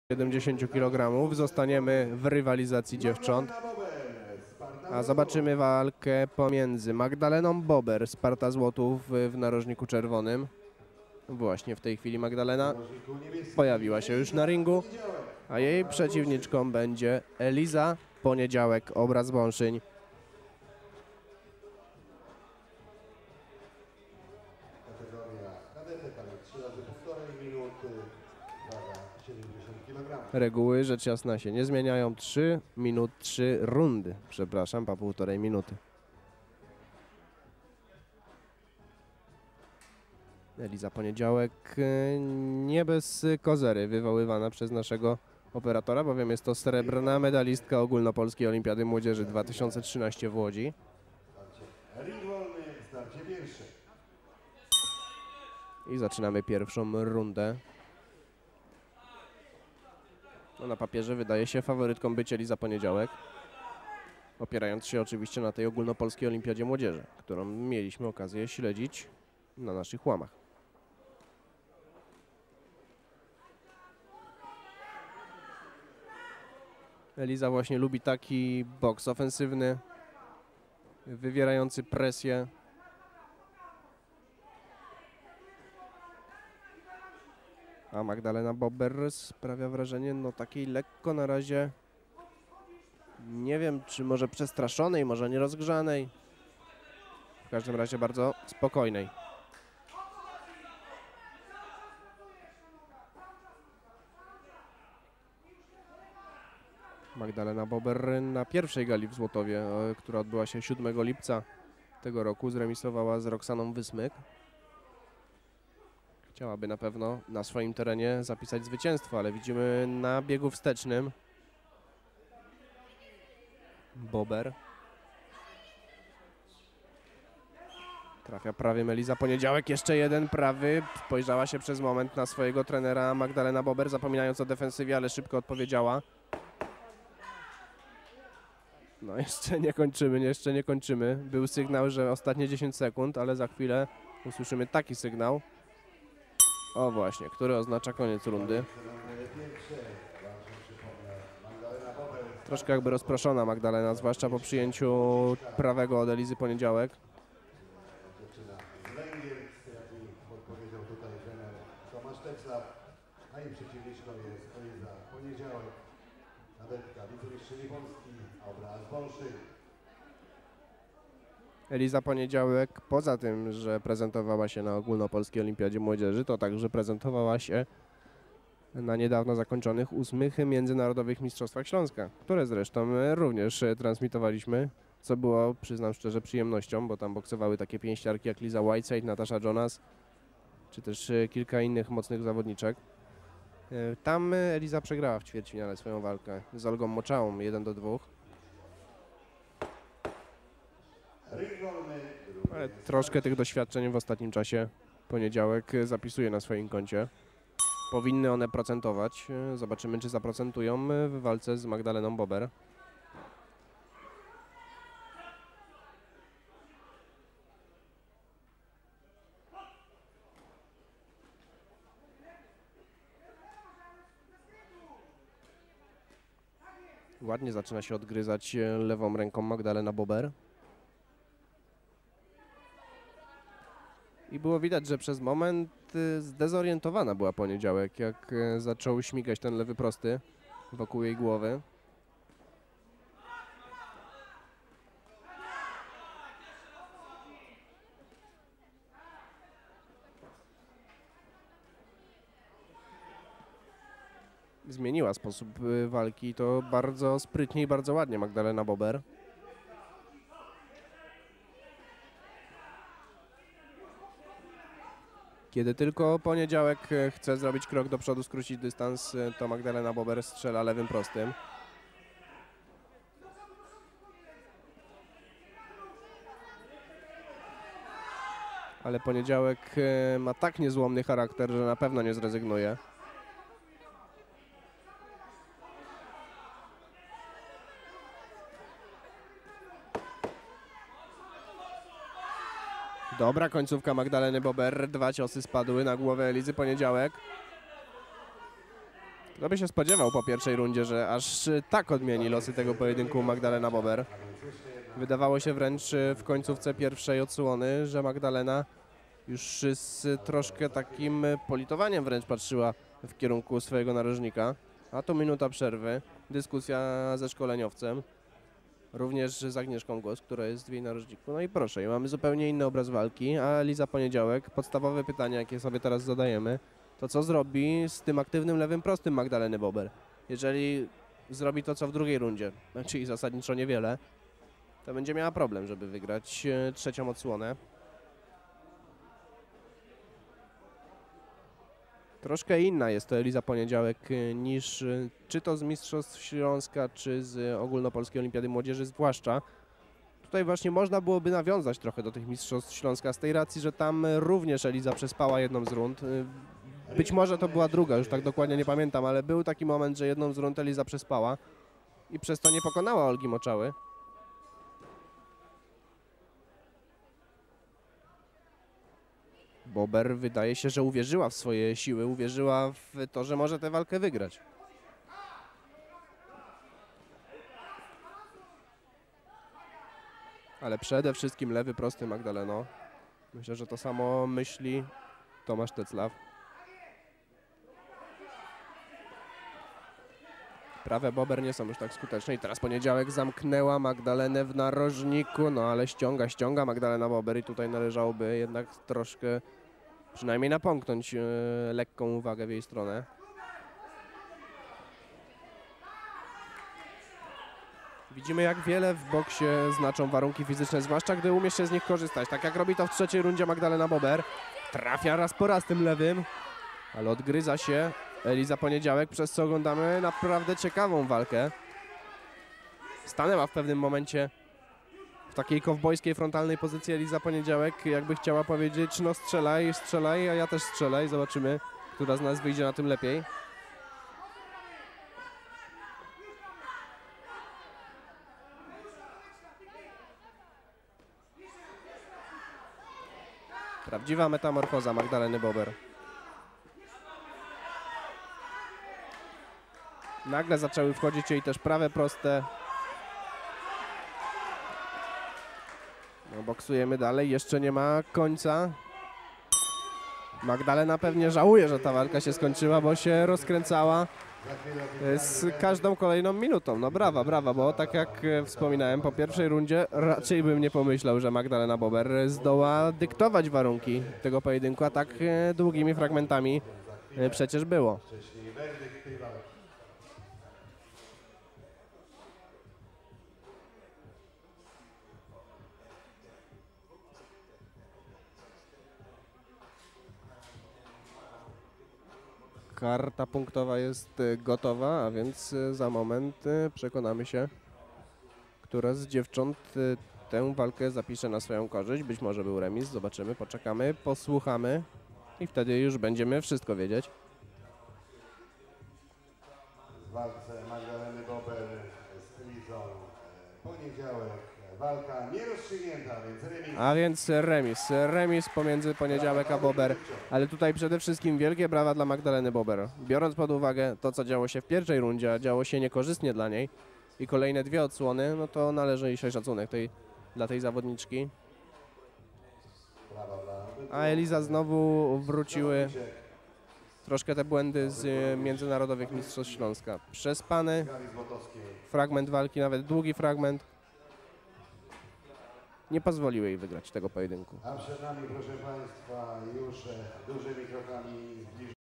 Eliza Poniedziałek vs Magdalena Bober
- Największy portal bokserski w Polsce przedstawia walkę kobiet Eliza Poniedziałek vs Magdalena Bober